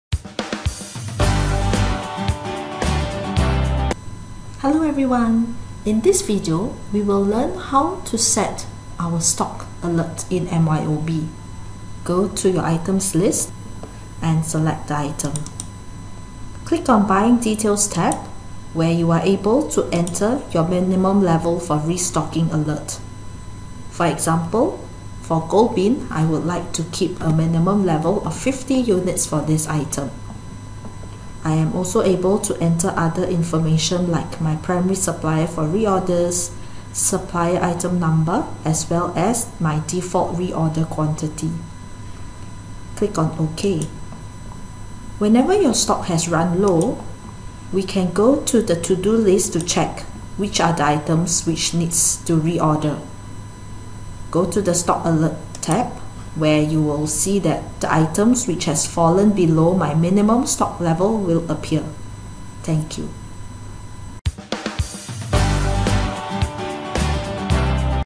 How to set restocking alert in MYOB Accounting
This video teaches you how to set alert for items whose stock quantity have fallen below the minimum level required.